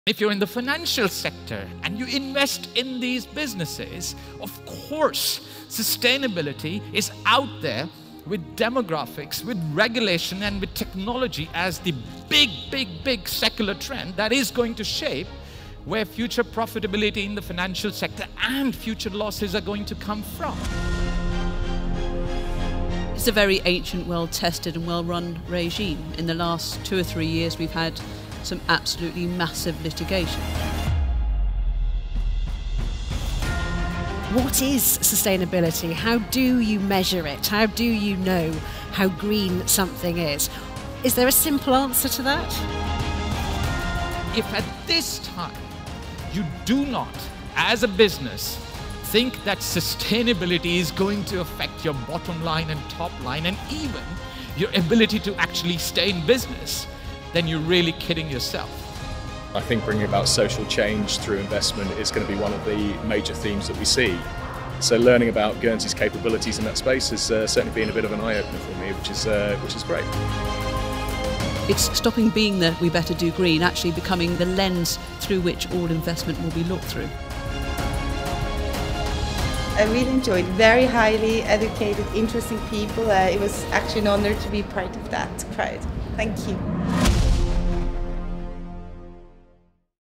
Private Wealth Forum 2019
As we prepare to move into a new decade and against the backdrop of a progressively uncertain globe, the values of substance, stability and sustainability move increasingly to the fore for the servicing of global private wealth. Reputational issues have become so significant that they are the determining factor of locational choice driving moves on regulation and transparency unimagined just a decade ago. All of this and more was debated at the 2019 Guernsey Private Wealth Forum.